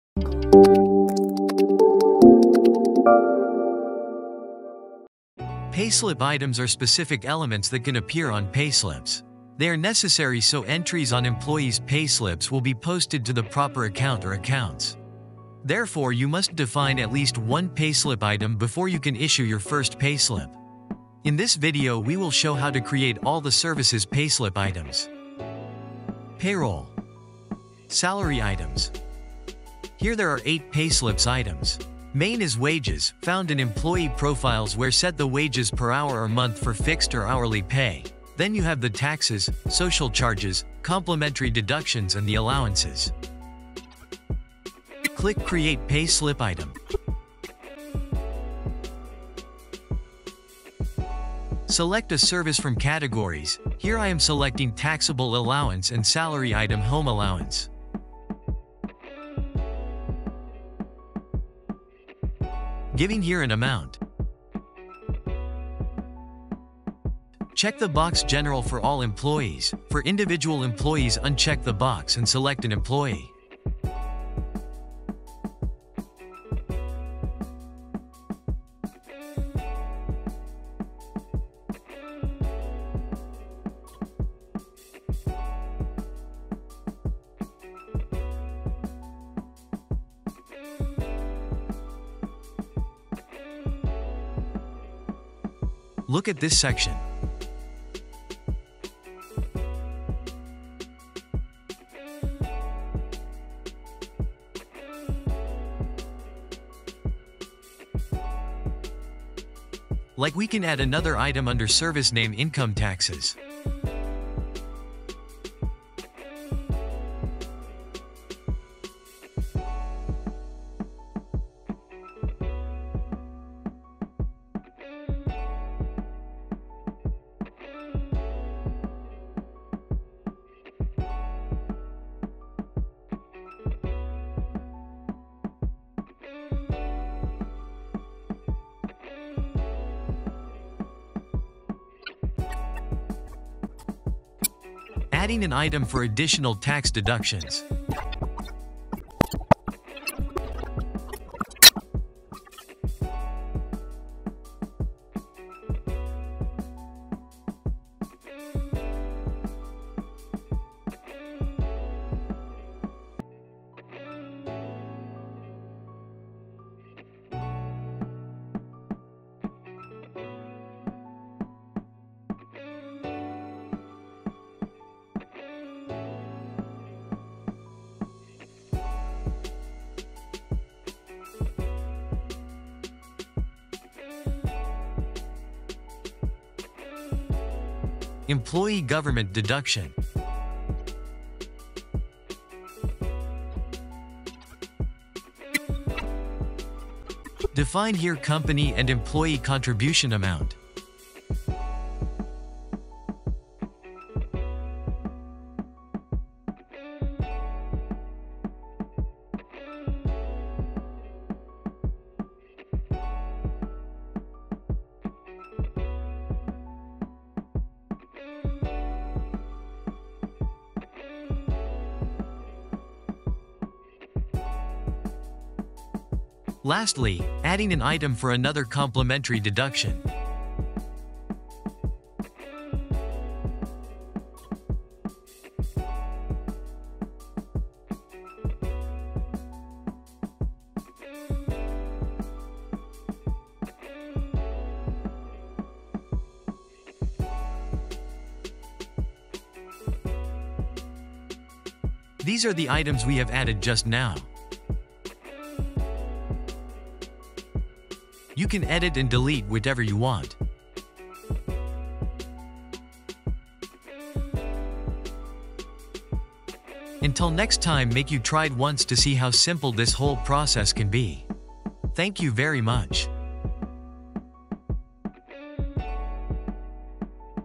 Create Payslip Items for all Services | Omada HR Payroll
Payslip items are specific elements that can appear on payslips.

They are necessary so entries on employees payslips will be posted to the proper account or accounts.

Therefore you must define at least one payslip item before you can issue your first payslip.

In this video we will show how to create all the services payslip items.

Payroll.

Salary Items.

Here there are eight payslips items.

Main is wages, found in employee profiles where set the wages per hour or month for fixed or hourly pay, then you have the taxes, social charges, complementary deductions and the allowances.

Click create pay slip item.

Select a service from categories, here i am selecting taxable allowance and salary item home allowance.

Giving here an amount.

Check the box general for all employees, for individual employees uncheck the box and select an employee.

Look at this section.

Like we can add another item under service name income taxes.

Adding an item for additional tax deductions.

Employee government deduction.

Define here company and employee contribution amount.

Lastly, adding an item for another complementary deduction.

These are the items we have added just now.

You can edit and delete whatever you want.

Until next time make you tried once to see how simple this whole process can be.